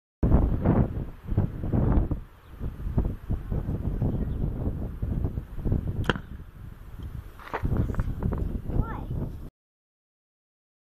braden 5 years old shooting his new bow
my son got this bow for his bday 4-10-11 and he was out shooting it. He loves it!